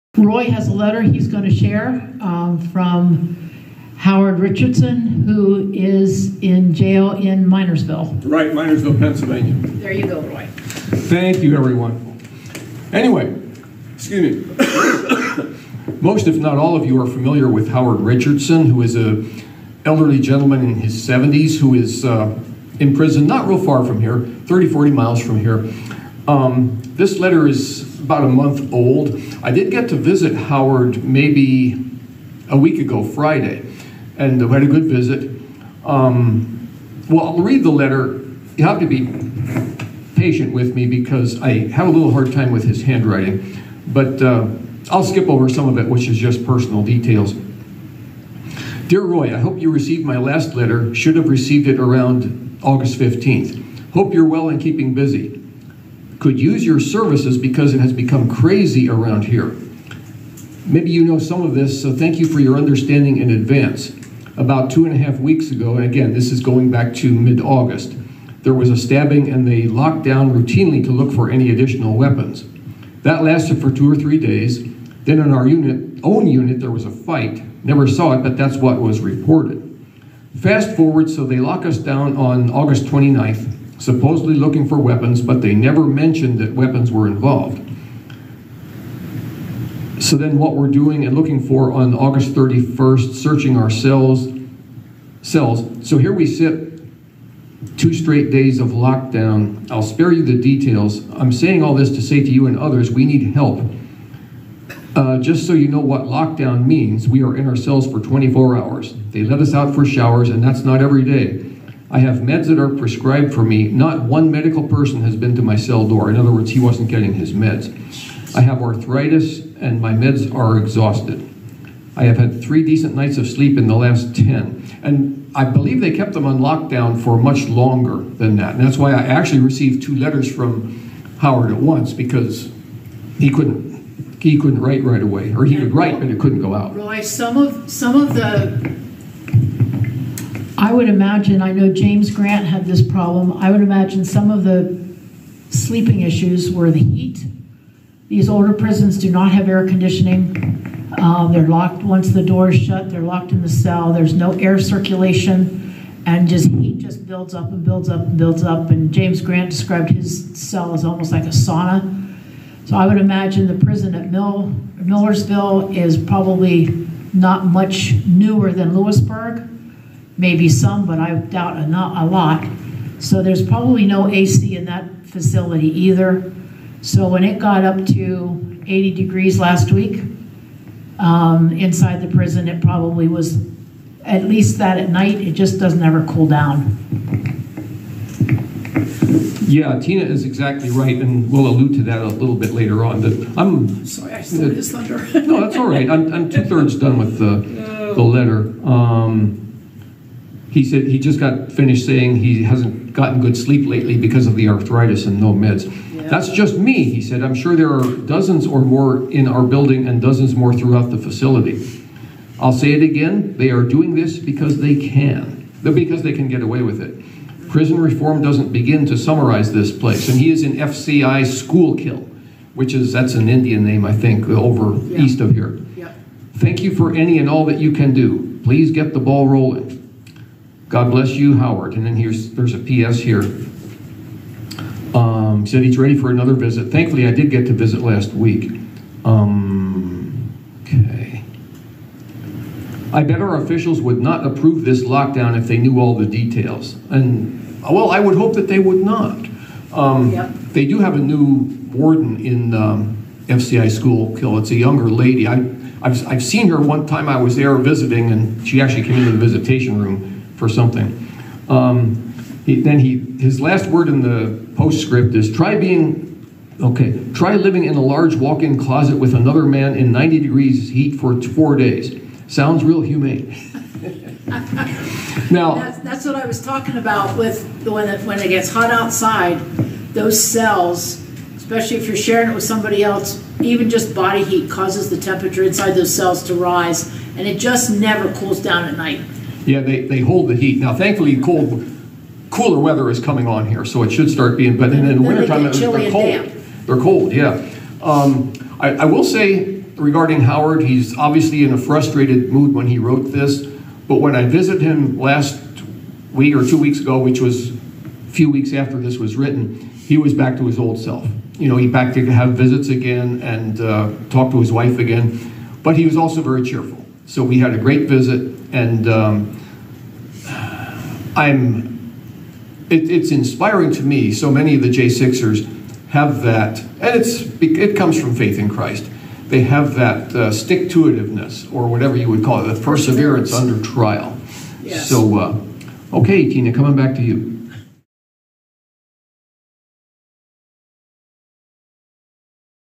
Update on Political Prisoner Howard Richardson
Roy reads Howards letter he received at the end of August and reports on his last recent in person visit. Good news to report...
Howard Richardson (Sentenced to 46 Months in Prison)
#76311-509
FCI Schuylkill